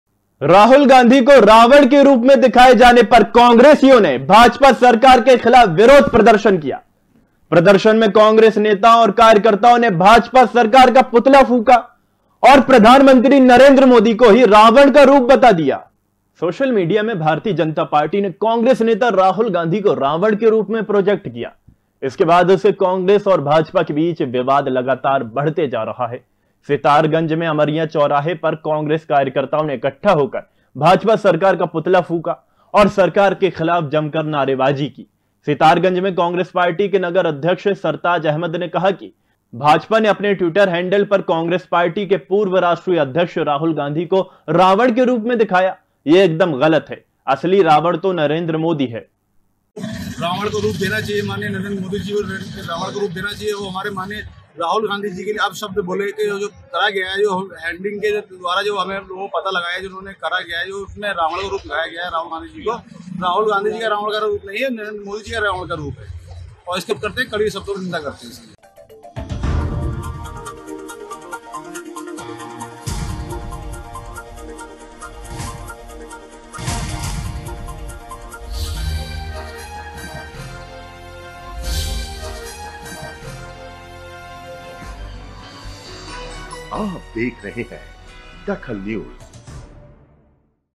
DAKHAL NEWS कांग्रेस ने भाजपा सरकार के खिलाफ किया प्रदर्शन
कांग्रेस ने भाजपा सरकार के खिलाफ किया प्रदर्शन
राहुल गांधी को रावण बताये जाने का किया विरोध